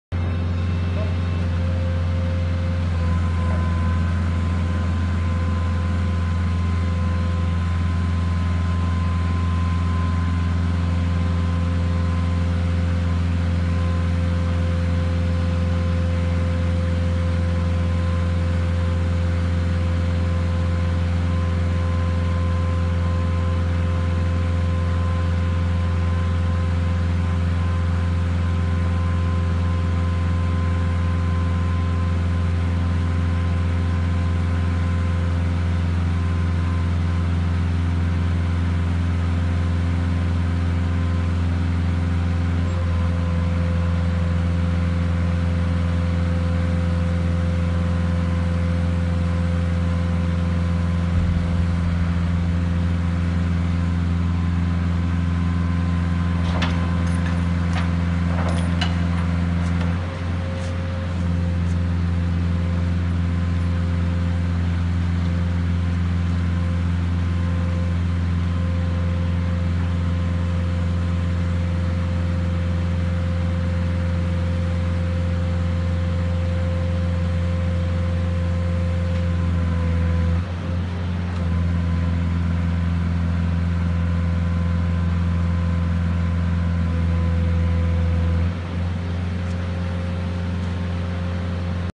Megalift SLT
Megalift SLT (side loading trailer) off loading container